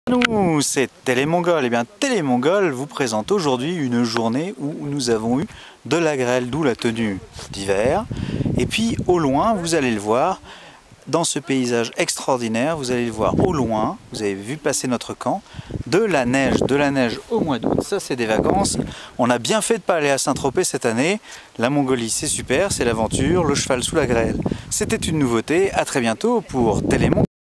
Télé mongol: trek à cheval, après la grêle
Assistés de yacks pour porter les tentes, nous nous engageons dans un trek en autonomie de 4 jours à cheval. Beau temps et intempéries s'alternent dans des paysages magnifiques aux confins de la vallée d'Orkhon, autour des huits lacs.